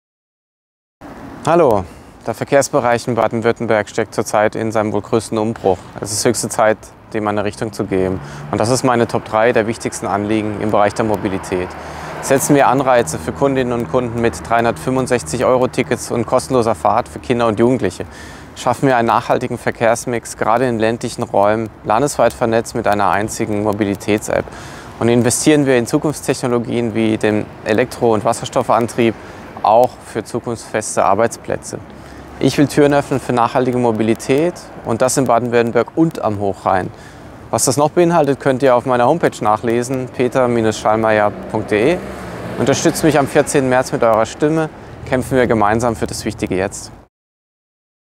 Landtagswahl 2021: Mein Programm #4
Am 14. März sind Landtagswahlen 🗳. Wofür die SPD und ich kämpfen ✊, stelle ich euch in den kommenden Wochen 🗓 hier vor.

‼ Heute: Türen öffnen für nachhaltige Mobilität. Film ab! 🎬